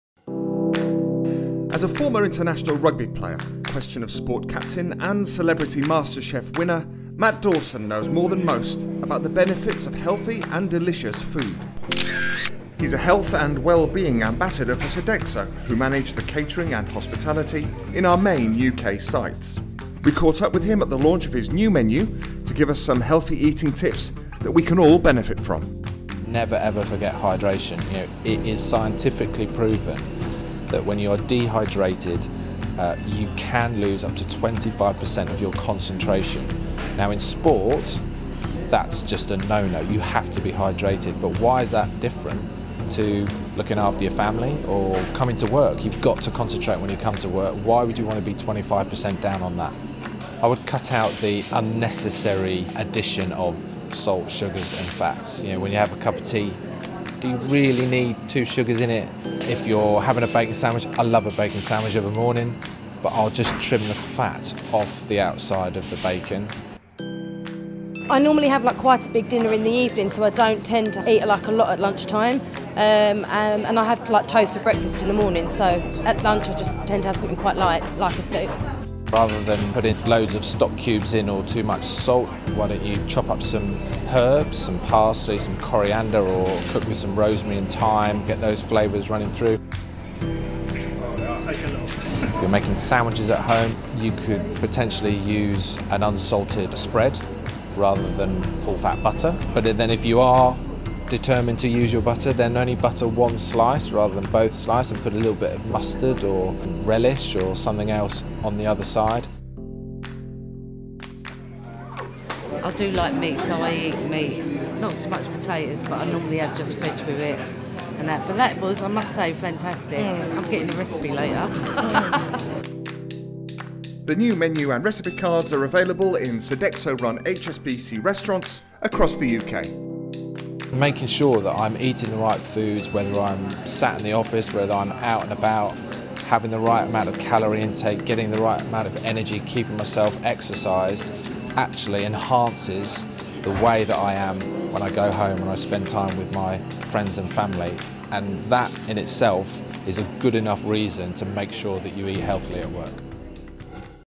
Matt Dawson on health & wellbeing
Sodexo's well-being ambassador Matt Dawson talks to us about healthy eating at the launch of a Sodexo healthy eating initiative and gives some top tips on healthy eating.